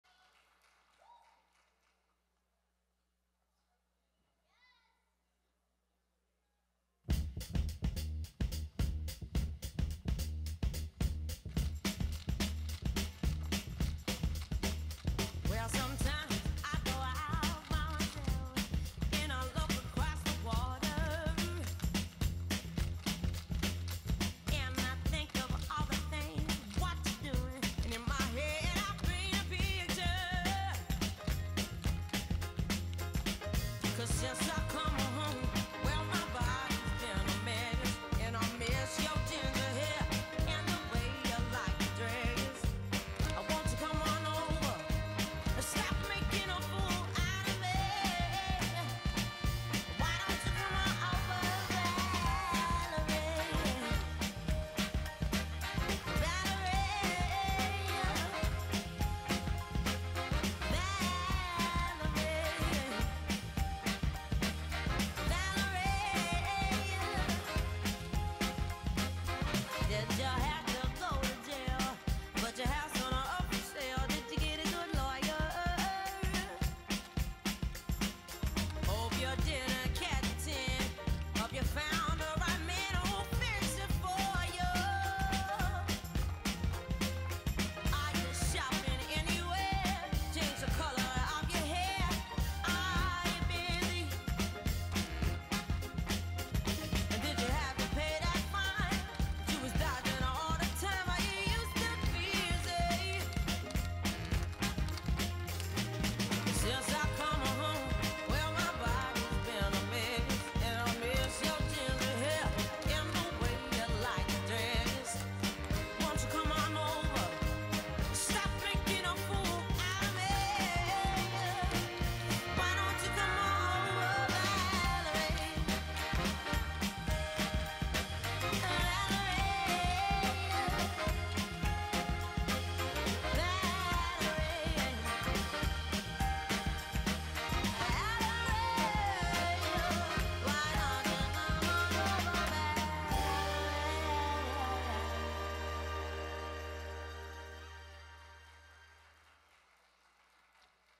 Valerie - Teen Tap - April 2017
from Star Systems Dance Competition
Dreams Dance Academy
Choreographed by Jay Fagan, rehearsed by Jenny O'Brien